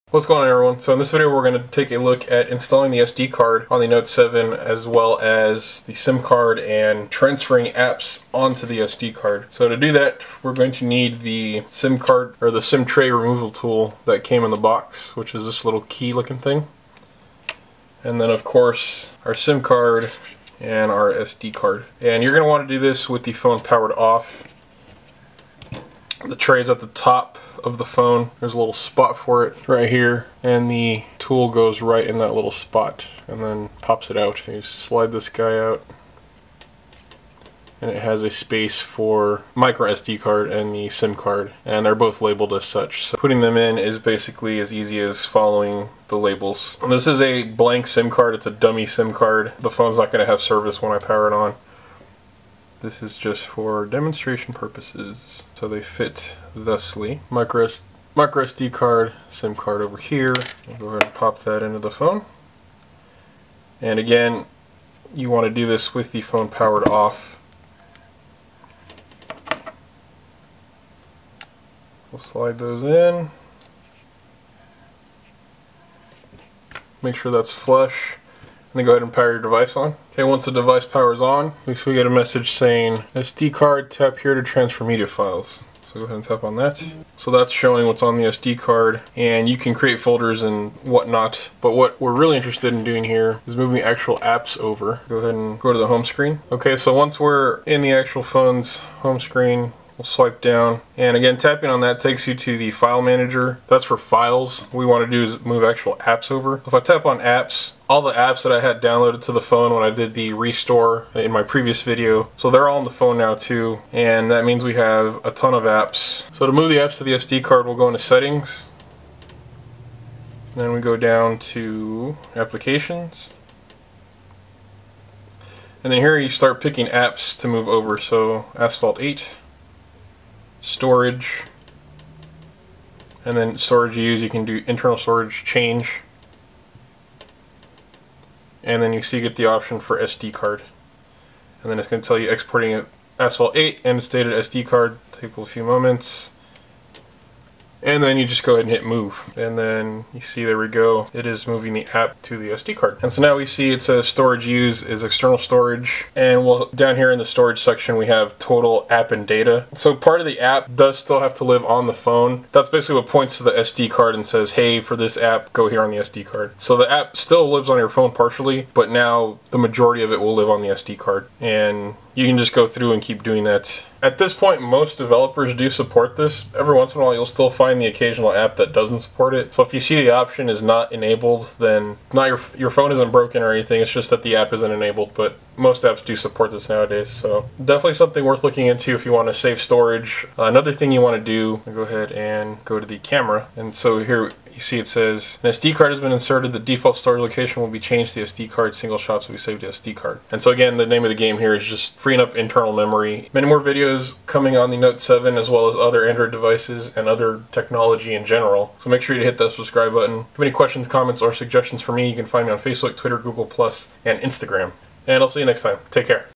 Samsung Galaxy Note 7 Transfer Apps to SD Card
The Samsung Galaxy Note 7 supports SD cards and one of the nice features of having an SD card, is the ability to offload apps and pictures to the SD.  In this video I'll show you how to move your apps over and show that your camera will begin using the SD card.